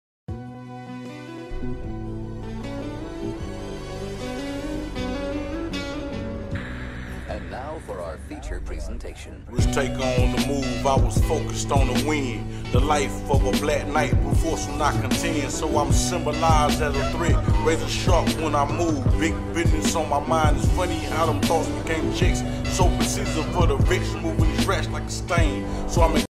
THE COMEBACK (KING KILLA)
Sneak peek of the comeback of king KILLA from bomar st